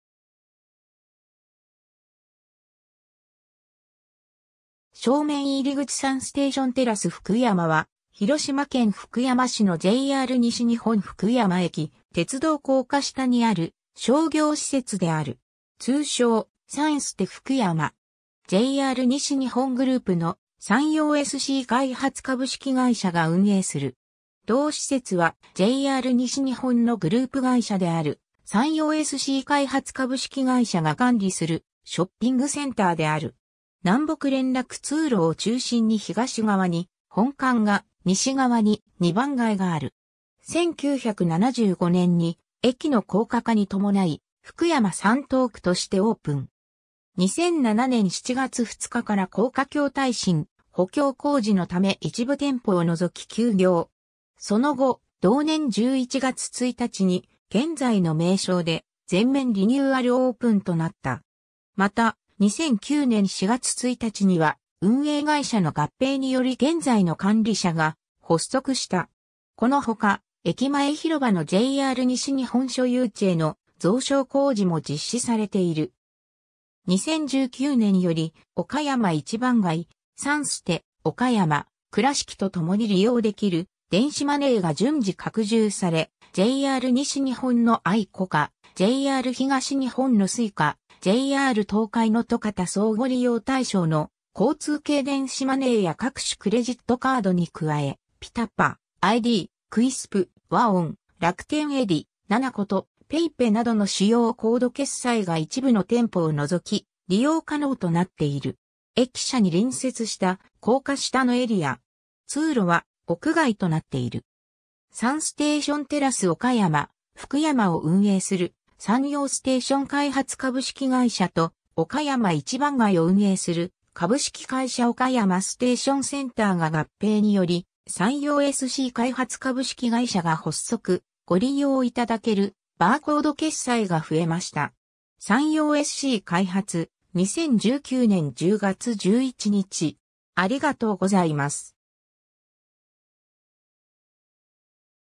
サンステーションテラス福山
正面入り口（2019年11月撮影） サンステーションテラス福山（サンステーションテラスふくやま、英語表記：SUN STATION TERRACE FUKUYAMA）は広島県福山市のJR西日本福山駅鉄道高架下にある商業施設（駅ナカ）である。
通称「さんすて福山」。
JR西日本グループの山陽SC開発株式会社が運営する。
 同施設はJR西日本のグループ会社である山陽SC開発株式会社が管理するショッピングセンターである。
南北連絡通路を中心に東側に「本館」が、西側に「2番街」がある。
 1975年（昭和50年）に駅の高架化に伴い「福山サントーク」としてオープン。
2007年（平成19年）7月2日から高架橋耐震補強工事のため一部店舗を除き休業。
その後、同年11月1日に現在の名称で全面リニューアルオープンとなった。
また、2009年（平成21年）4月1日には運営会社の合併により現在の管理者が発足した。
この他、駅前広場のJR西日本所有地への増床工事も実施されている。
 2019年より岡山一番街、さんすて岡山・倉敷とともに利用できる電子マネーが順次拡充され、JR西日本のICOCA、JR東日本のSuica、JR東海のTOICA他相互利用対象の交通系電子マネーや各種クレジットカードに加え、PiTaPa、iD、QUICPay、WAON、楽天Edy、nanacoと、PayPayなどの主要コード決済が一部の店舗を除き利用可能となっている。
 駅舎に隣接した高架下のエリア。
通路は屋外となっている。
 ^ サンステーションテラス岡山・福山を運営する山陽ステーション開発株式会社と、岡山一番街を運営する株式会社岡山ステーションセンターが合併により山陽SC開発株式会社が発足 ^ ご利用いただけるバーコード決済が増えました！ - 山陽SC開発、2019年10月11日。